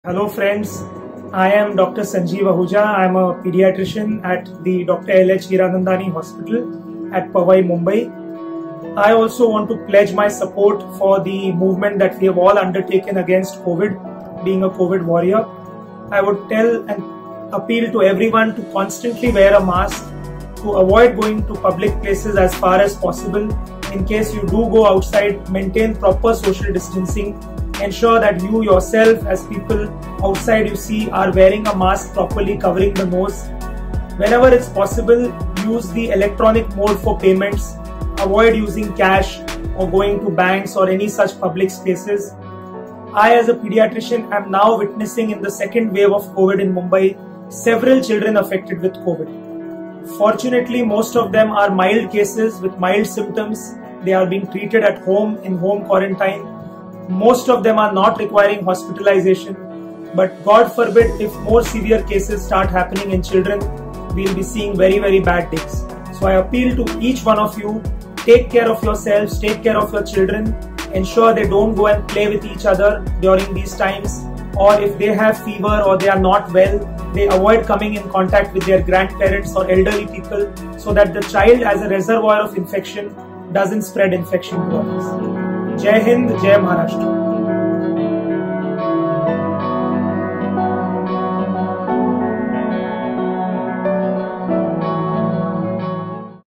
Dr Sanjeev Ahuja | Paediatrician | Dr L H Hiranandani Hospital | The Professional Times
Dr Sanjeev Ahuja - Paediatrician at Dr L H Hiranandani Hospital, Powai, Mumbai pledging to the people to take full precautions and also talking about how to save your children at this difficult time.

The Professional Times has come forward in support of the Government of Maharashtra's "Break The Chain" campaign against the deadly Corona Virus.